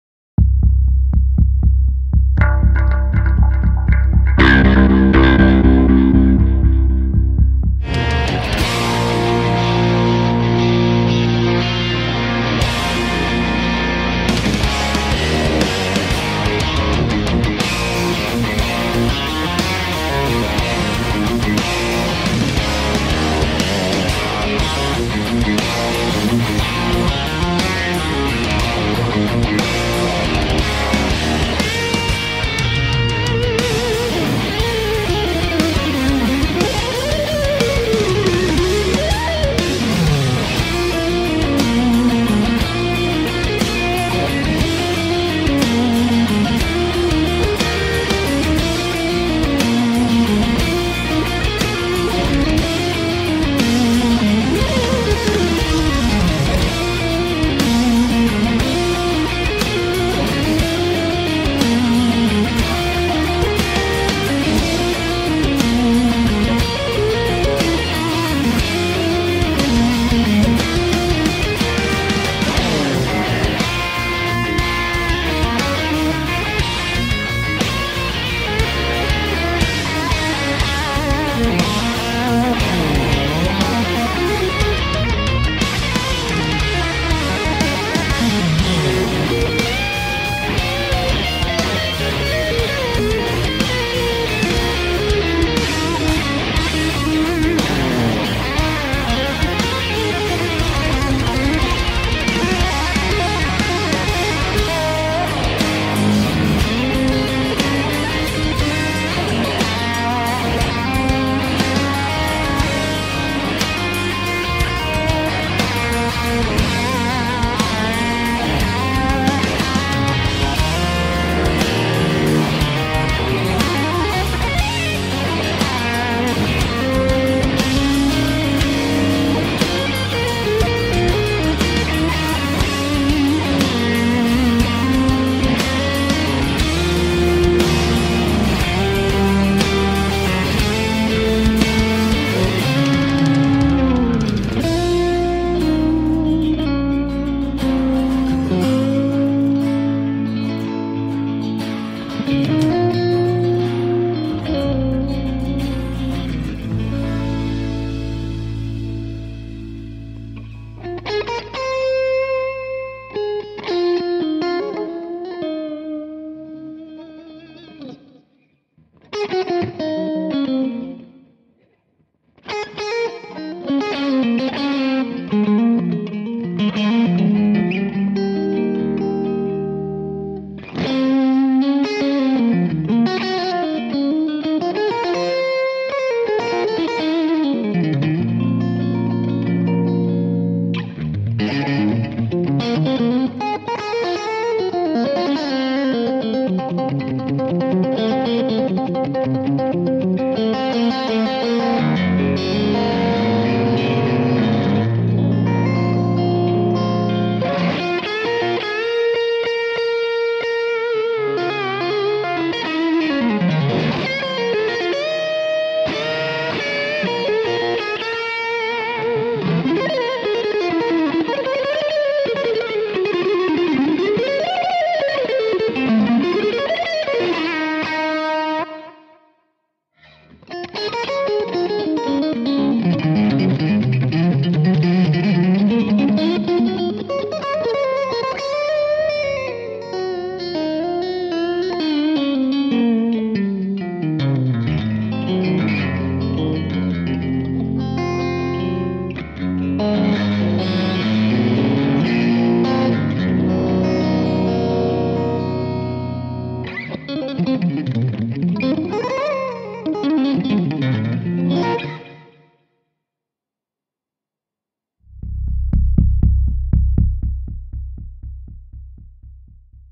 Kemper Profiler Performance - Nick Johnston
We have met Nick Johnston at NAMM 2017, thanks to the introduction of Keith Merrow. He is an incredibly versatile guitar player and we are extremely happy that he just sent us this video.